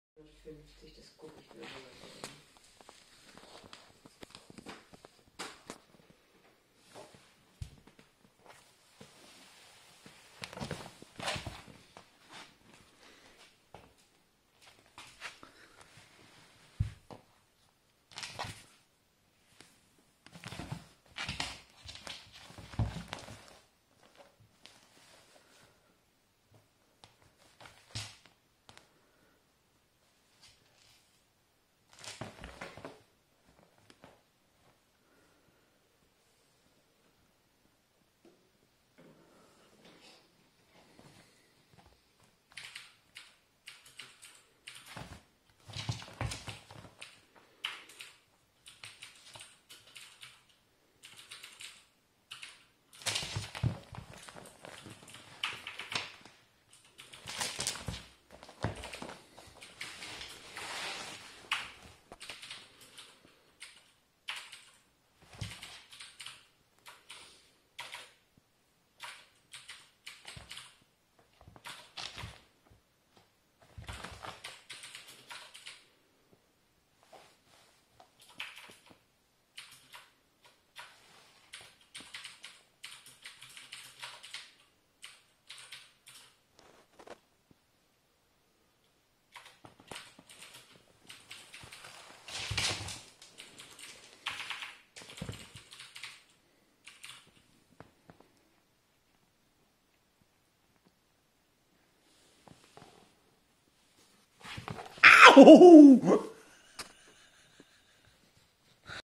Kitten vs. Rug
Kitten vs. Rug Rambunctious 4 Month Old Calico Kitty versus a rug.  Plus viscous attack!